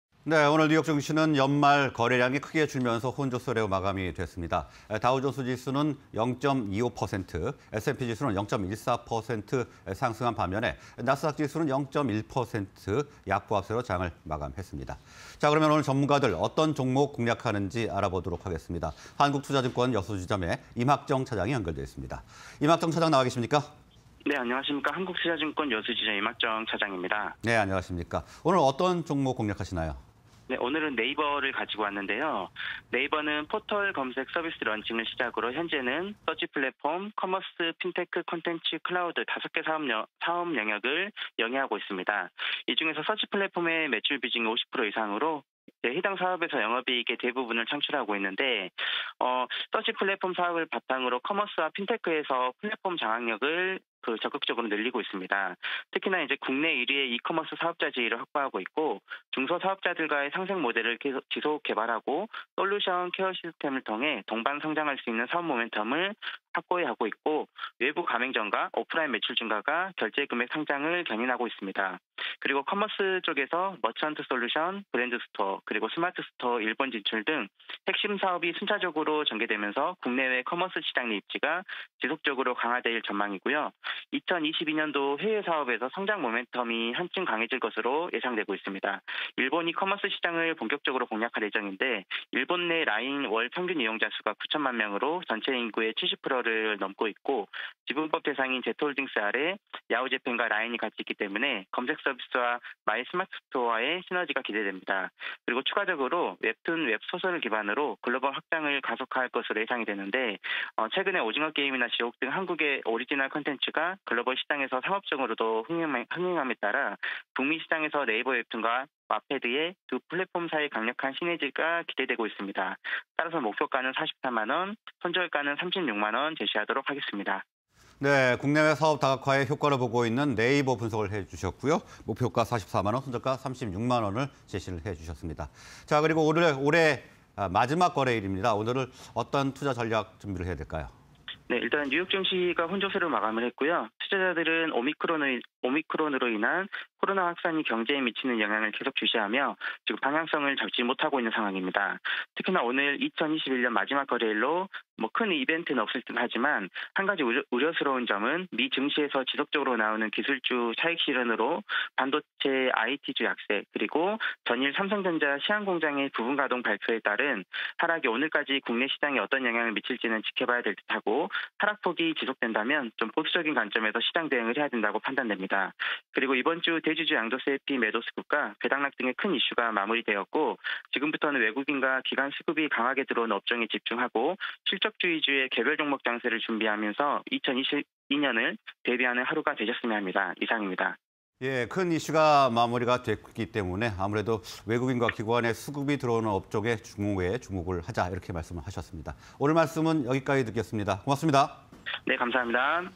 NAVER(035420)... 플랫폼 경쟁력 바탕 수익모델 성장_굿머니 공략주 (20211230)
NAVER(035420)
- 플랫폼 경쟁력 바탕 수익모델 성장
- 국내외 사업 다각화 효과
- 커머스와 콘텐츠 성장 주목